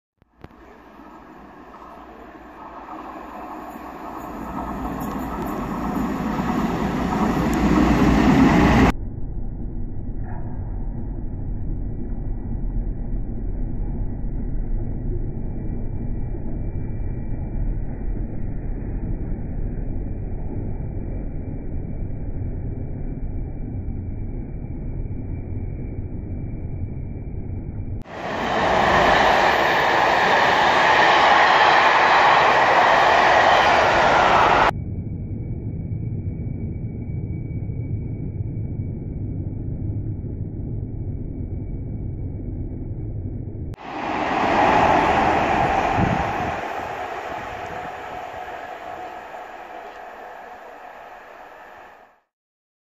46100 Royal Scott with The English Riviera Express at keynsham !!!
This was my first rail tour that I have seen since lockdown and what a start to it, pulled by 46100 Royal Scott and a class 47 at the rear through keynsham if you enjoy like and subscribe for more content like this. 😀😀🚂🚂